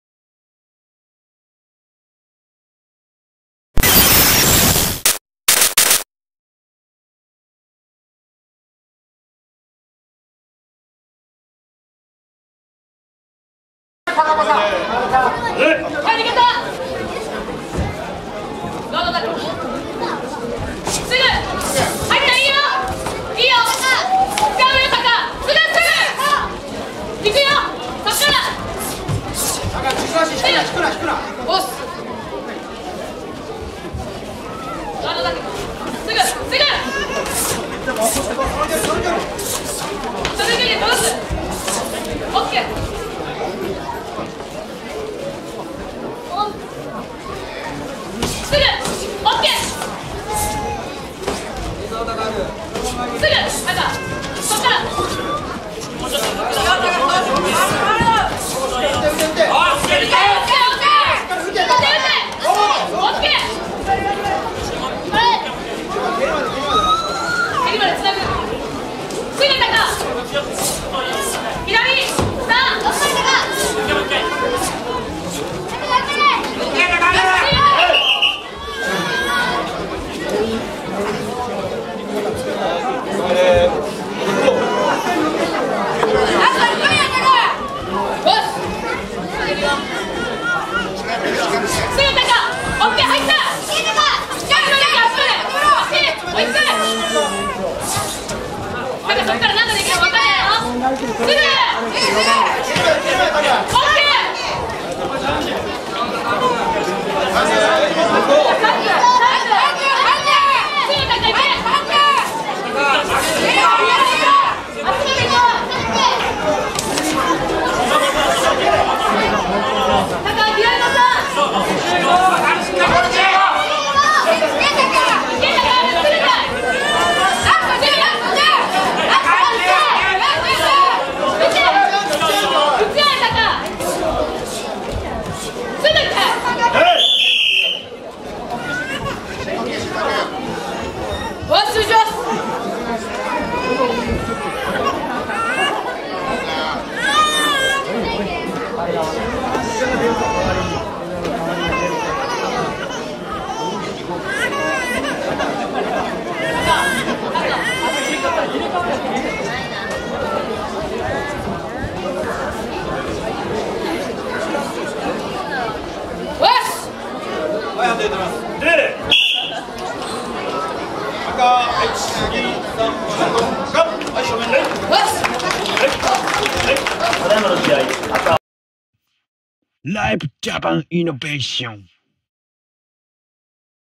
【空手大会】中学生男子 ＋50kg 第4回極CUP空手道交流大会 Karate junior high school student boys +50kg
第4回極CUP空手道交流大会の中学生重量級の部の決勝戦です‼

日にち：平成30年2月18日
場所：福岡県須恵町立武道館
録画：Li武 Japan Innovation（LIVE JAPAN INNOVATION）